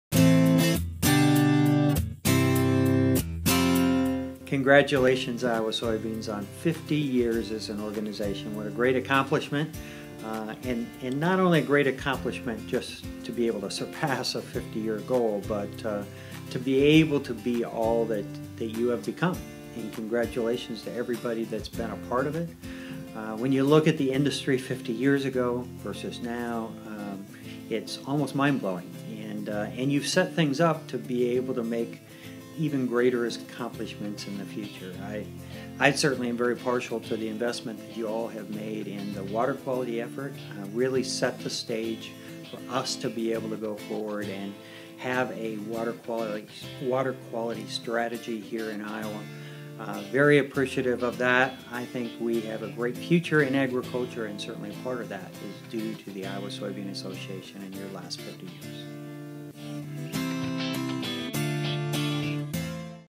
Congratulations on 50 years!
For 50 years Iowa farmers have worked to further the future of the soybean and it’s time to celebrate that journey. In the coming weeks watch as farmers, industry partners and others congratulate the Iowa Soybean Association on 50 years of expanding opportunities and delivering results for Iowa soybean farmers. We invite you to join in the celebration by following the posts and making your own congratulations video. Post your video using the hashtag CongratsISA.